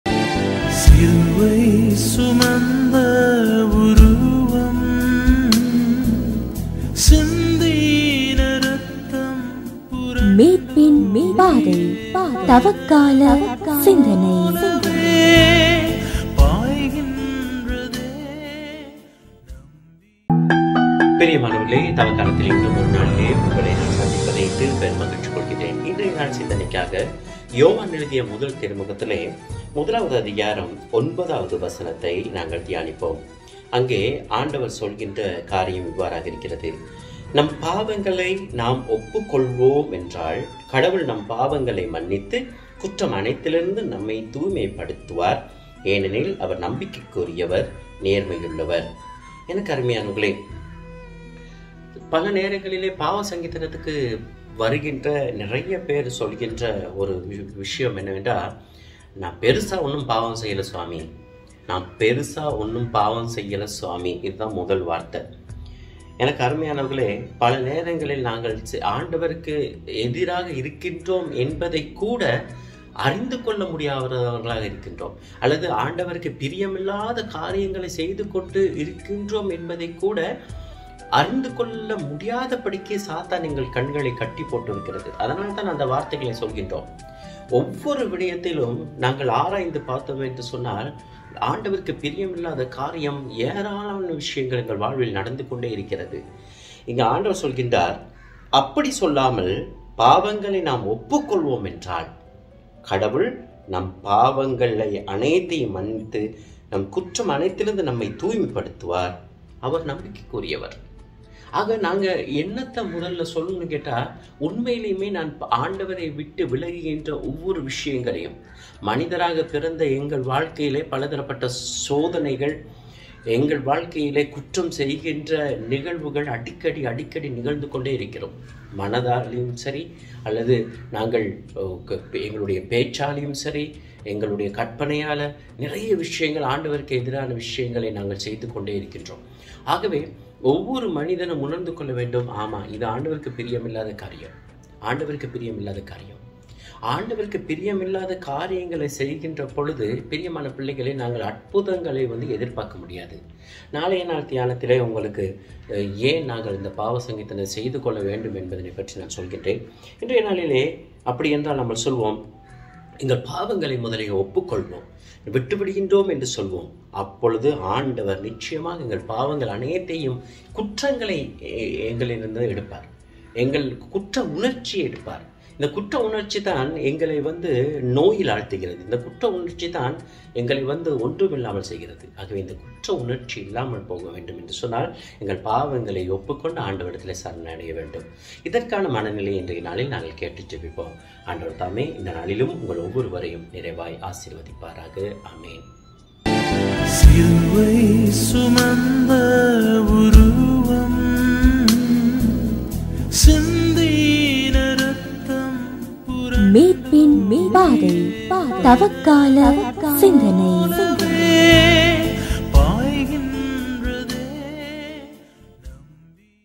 Lenten Thought | தவக்கால சிந்தனை | Fr. Anthony Fernando CMF | Claretian Communications
Lenten Thought  Published by Claretian Communications, Tambaram, Chennai. Indraya Irai Mozhi was given by Fr. Anthony Fernando CMF. Copyright  @Claretian Communications